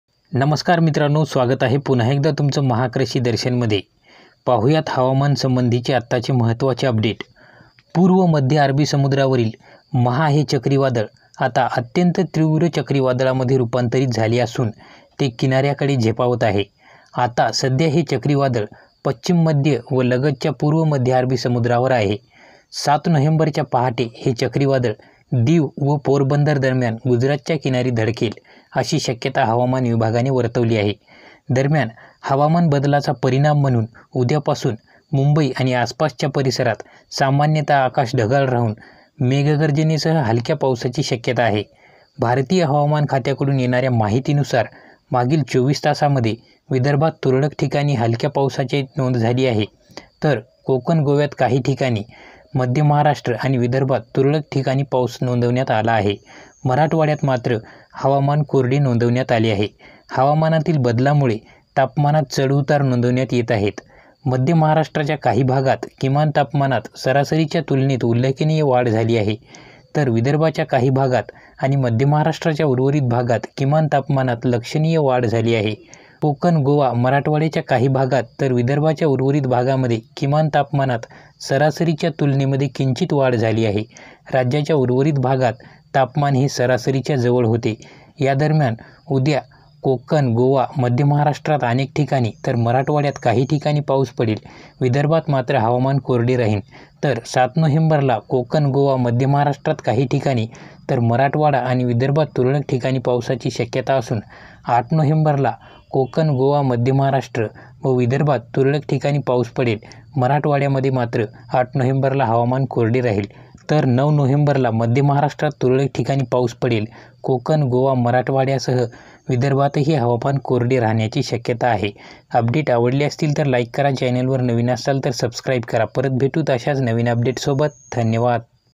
चक्रीवादळाची किणाऱ्याकडे झेप | Cyclone maha updates Maharashtra
हवामानाचा अंदाज महाराष्ट्र,गेल्या २४ तासांत विदर्भात तुरळक ठिकाणी हलक्या स्वरुपाच्या पावसाची नोंद झाली आहे. कोकण, गोव्यात काही ठिकाणी तर मध्य महाराष्ट्र आणि विदर्भात तुरळक ठिकाणी पाऊस नोंदविण्यात आला आहे. मराठवाड्यात हवामान कोरडे नोंदविण्यात आले आहे. हवामानातील बदलामुळे तापमानात चढ उतार नोंदविण्यात येत आहेत